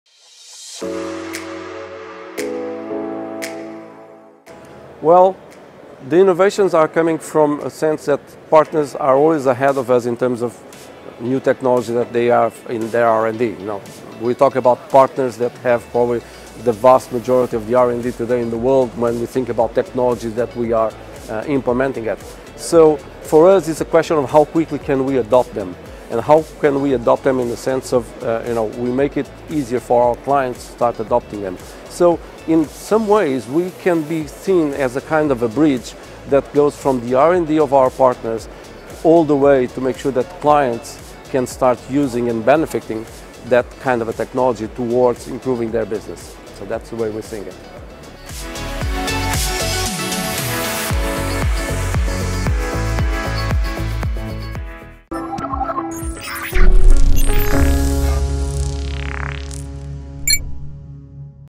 How Will New Partnerships Drive Innovation in the Industry? | Interview with Pedro Silva at MWC
Pedro Silva, Executive Director of Partnerships at Celfocus, discusses how he expects new partnerships to drive innovation in the telecoms industry.